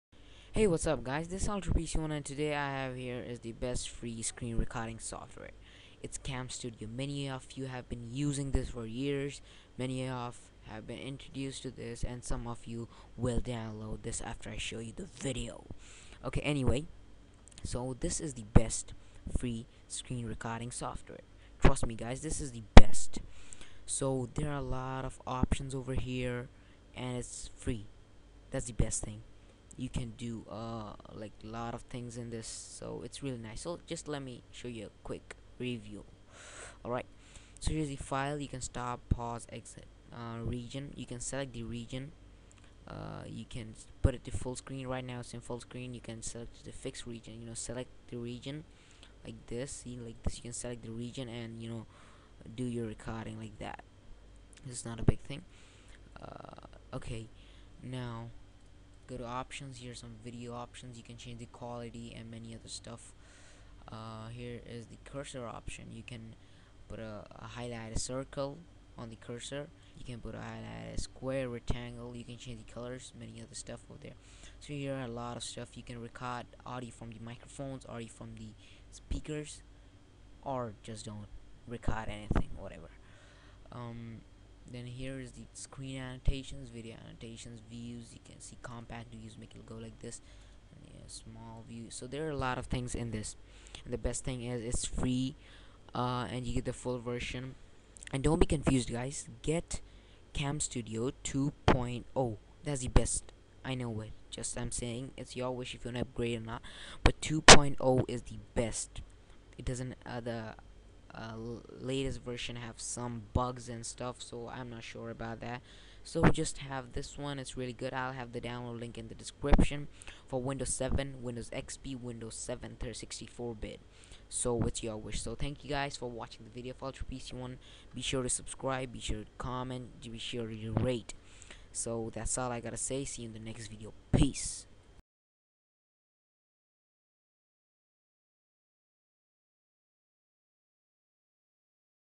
Best Free screen recording software
Hey guys in this video i will show you the which is the best free screen recording software

For all windows